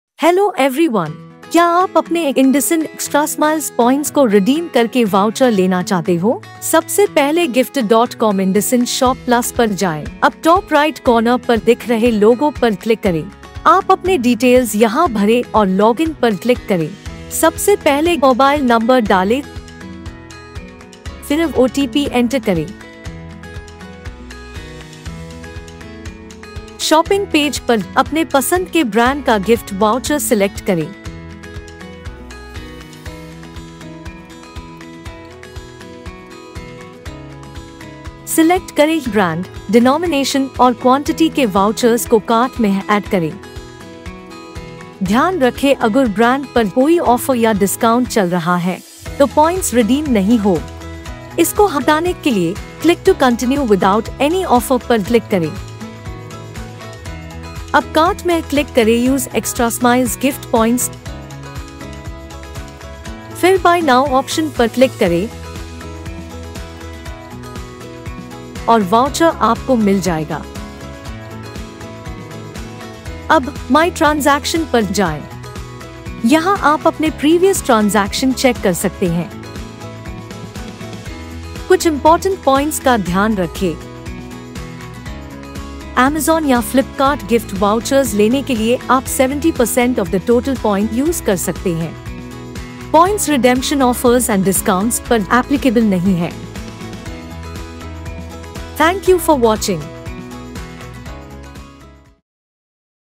Xtrasmiles Redemption Process in Hindi | GyFTR
Hi Guys, now you can check-out the detailed steps of redeeming Xtrasmiles Gift Voucher